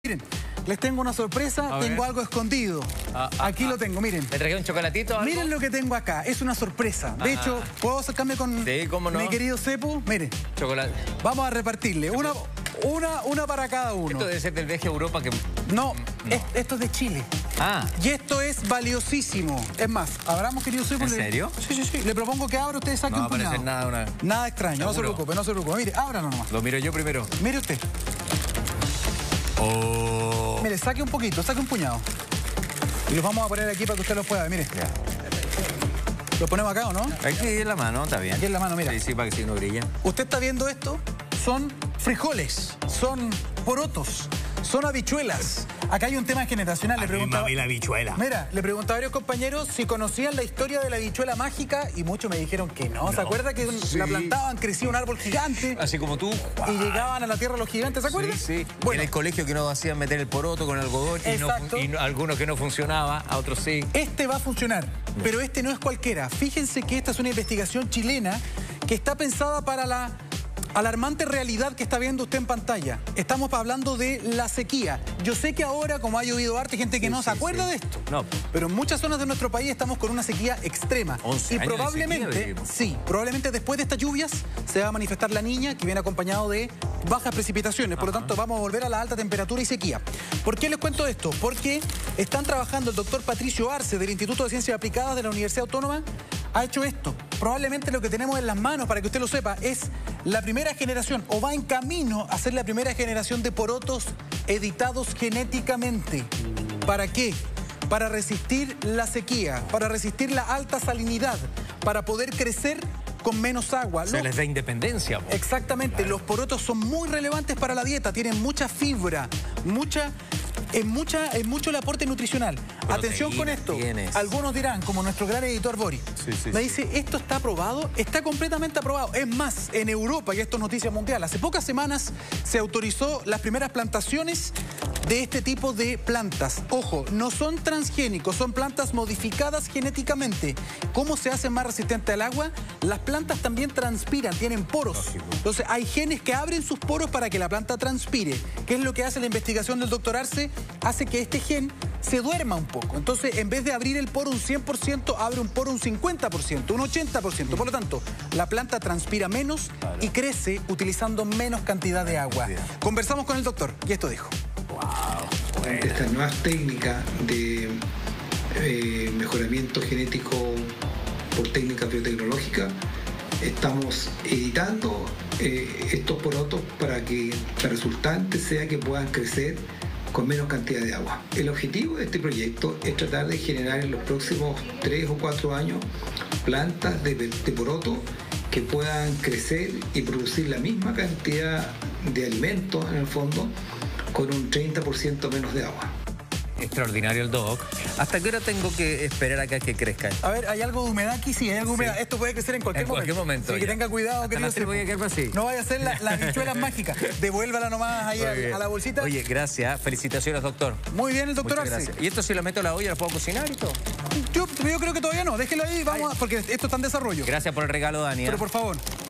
¿Porotos que resisten la sequía? La innovación chilena que ayuda al planeta
Un grupo de científicos chilenos desarrollaron un poroto modificado genéticamente que es resistente a la sequía.